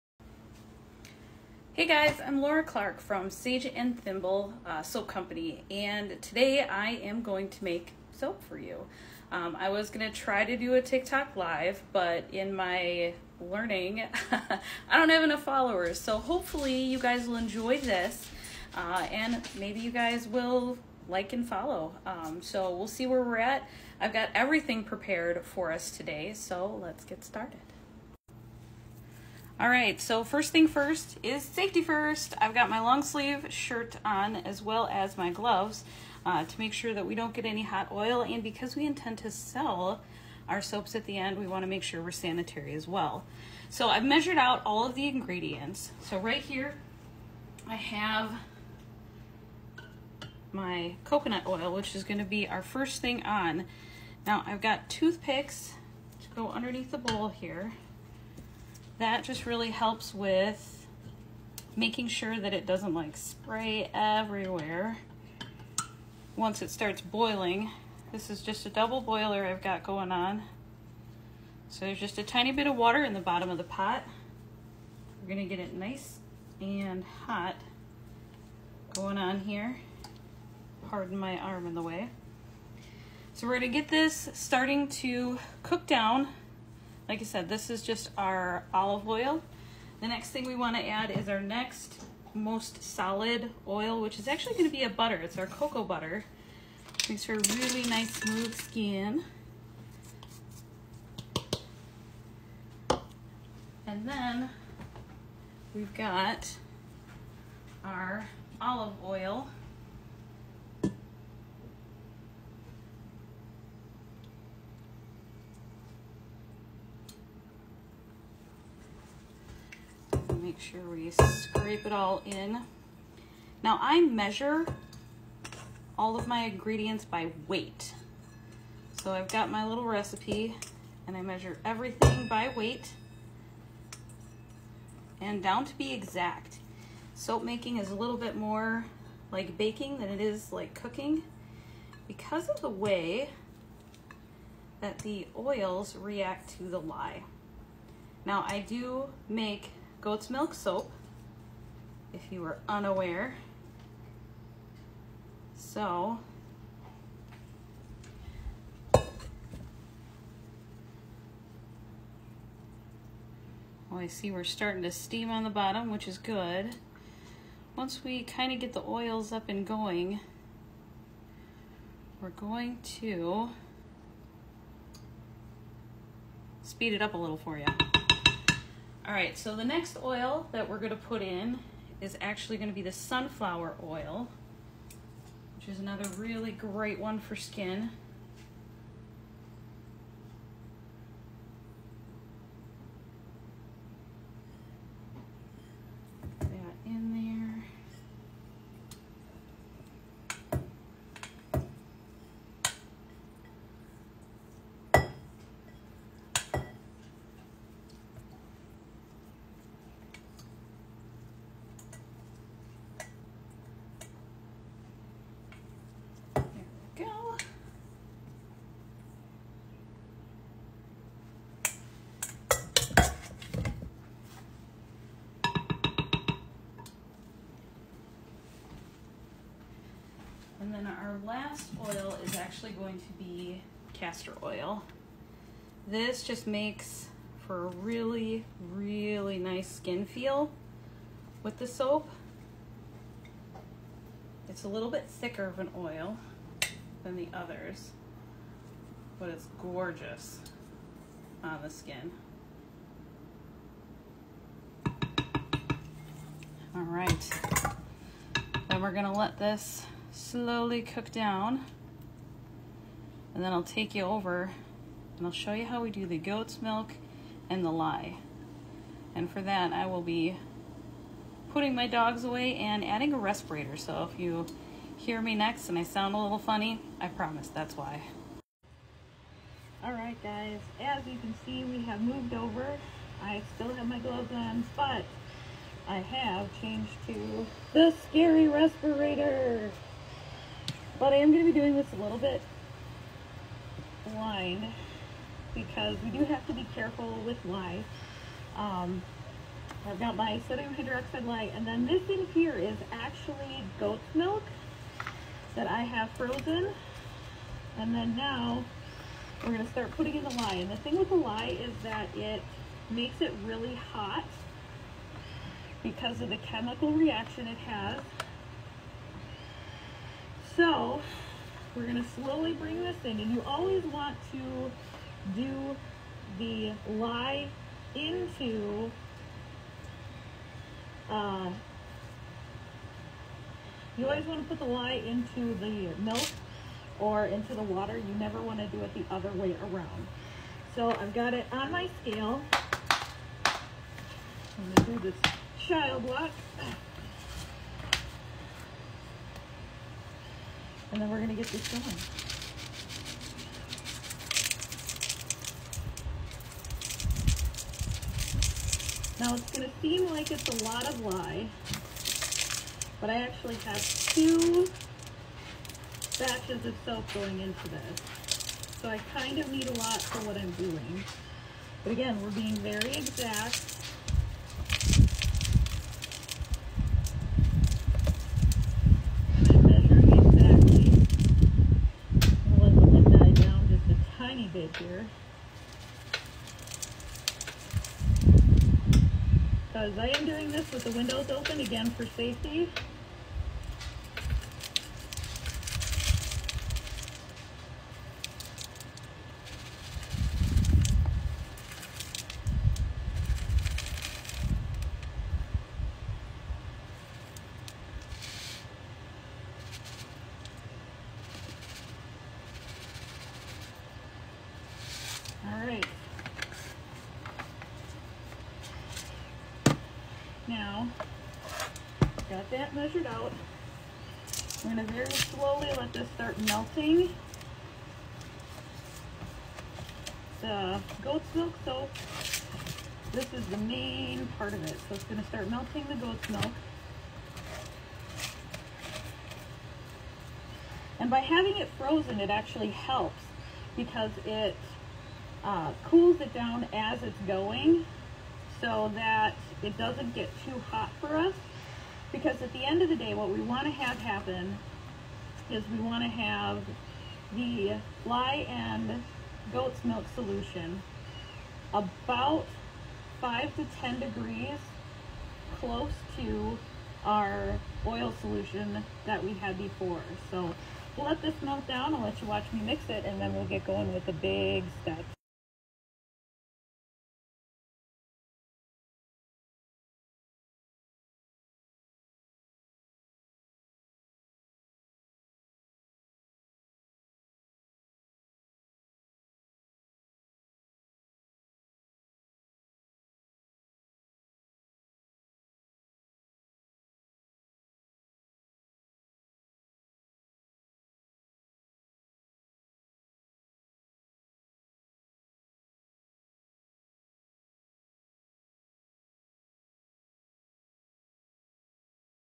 Part 1 Making Lymphatic Support Soap!
Part 1 of making Lymphatic Support soap! My hottest selling soap is complicated to make but smells wonderful! No dyes or artificial additives, just beautiful handcrafted goodness with the smells of rosemary, cypress and my favorite bergamot! Join me at I start the process!

Socials:
Sageandthimble.com

Organic Rosemary
Rosemary Essential Oil
Caster Oil
Olive Oil
Essential Oil Set
Coconut Oil